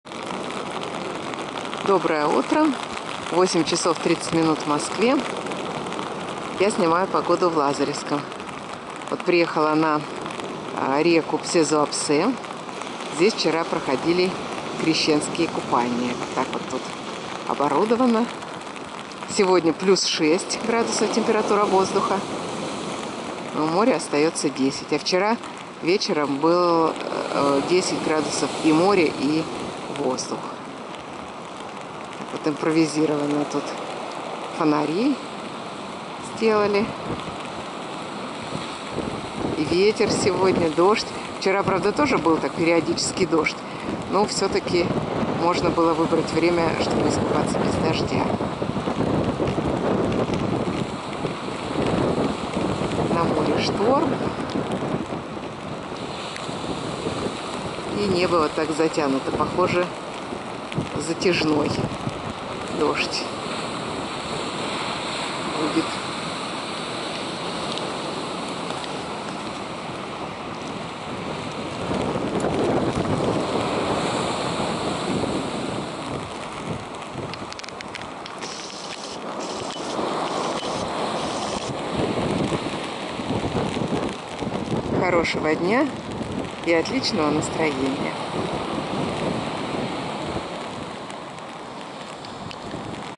Спасательный круг на Крещение! Купель на реке Псезуапсе. Лазаревское, январь 2016
Даже на Крещение без спасательного круга купаться нельзя. Устье реки Псезуапсе в Лазаревском. Вчера здесь была купель, сегодня я приехала сюда снять видео о погоде... дождь, t  +6°C, море  t  +10°C